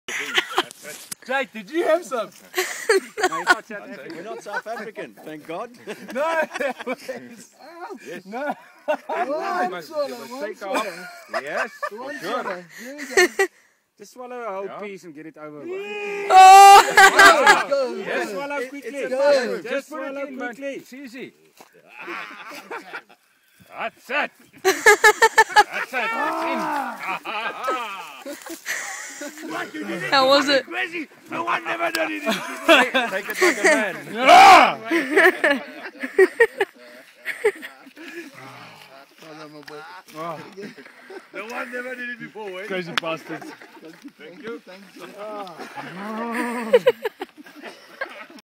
Testicle fiesta!
After shooting my first ever animal, i was then forced to eat the Bucks testicle! Not a nice experience at ALL!